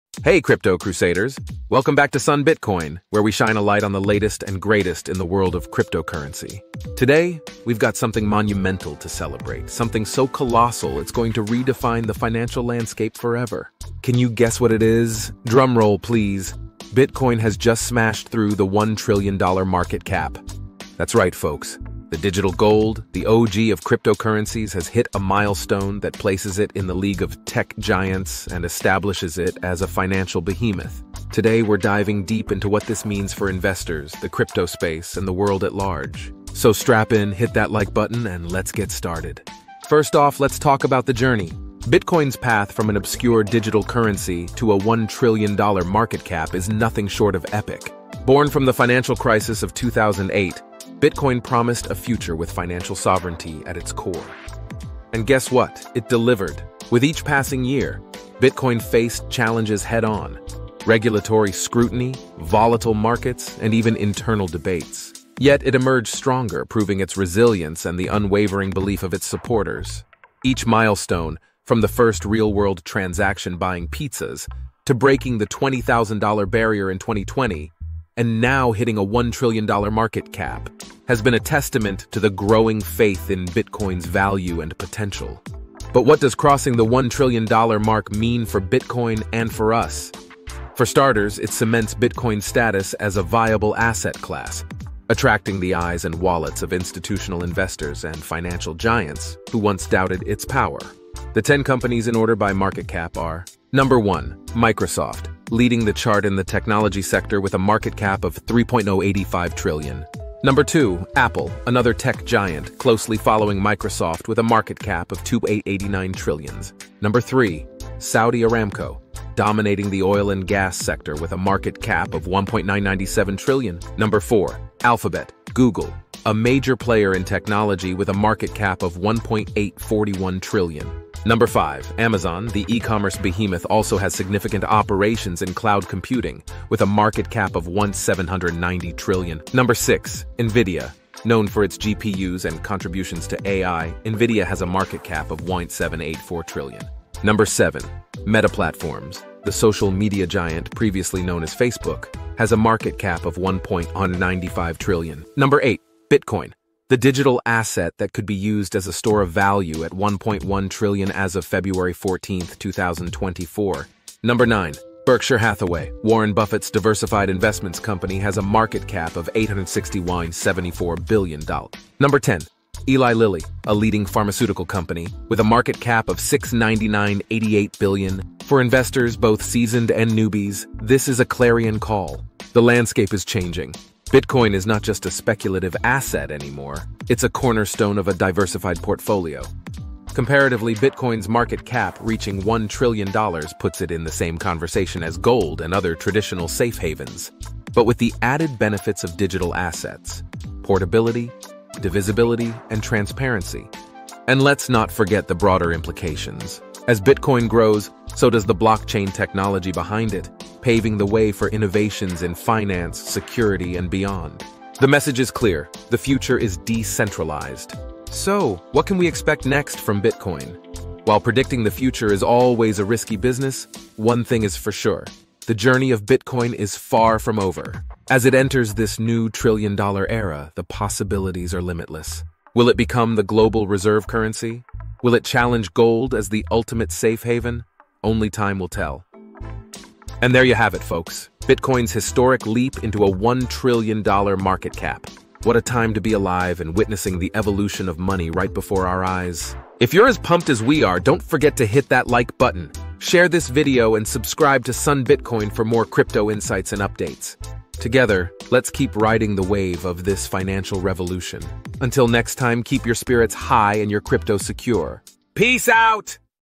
BITCOIN 1 TRILLION MARKET CAP
"Hey, Crypto Crusaders! Welcome back to Sunbitcoin, where we shine a light on the latest and greatest in the world of cryptocurrency. Today, we've got something monumental to celebrate, something so colossal it's going to redefine the financial landscape forever. Can you guess what it is? Drumroll, please...  Bitcoin has just smashed through the 1 trillion dollar market cap! .

"That's right, folks! The digital gold, the OG of cryptocurrencies, has hit a milestone that places it in the league of tech giants and establishes it as a financial behemoth. Today, we're diving deep into what this means for investors, the crypto space, and the world at large.